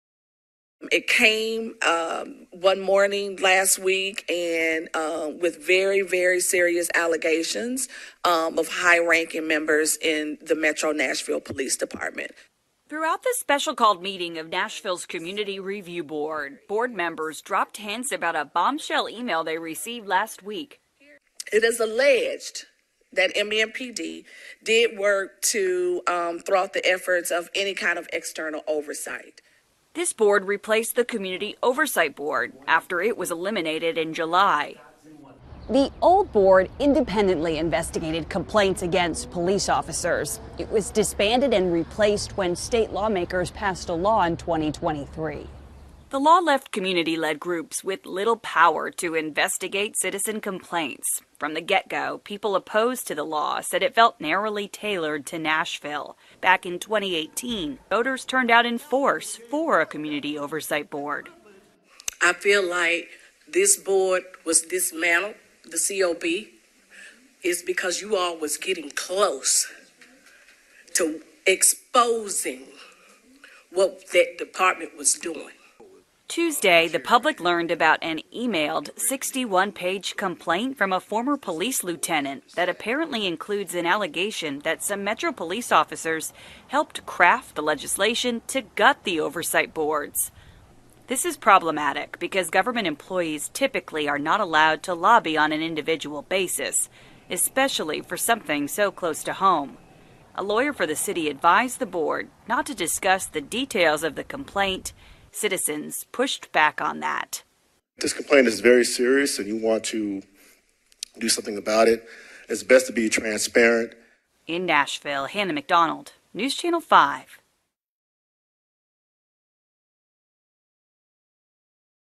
Former MNPD officer says some helped lawmakers abolish community board
A former lieutenant claims Metro Police officers helped write the state law that gutted police oversight boards.